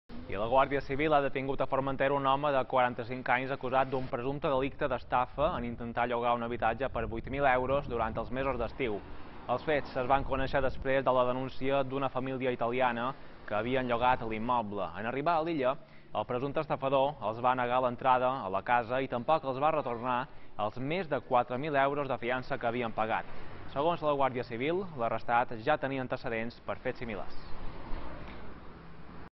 Detingut per estafar uns turistes amb un lloguer a Formentera
La Guàrdia Civil ha detingut a Formentera un home de 45 anys acusat d’un presumpte delicte d’estafa en intentar llogar un habitatge per 8.000 euros a una família italiana durant els mesos d’estiu. En arribar a l’illa, el presumpte estafador els va negar l’entrada a la casa i tampoc els va retornar els més de 4.000 euros de fiança que havien pagat. Segons la Guàrdia Civil, l’arrestat ja tenia antecedents per fets similars.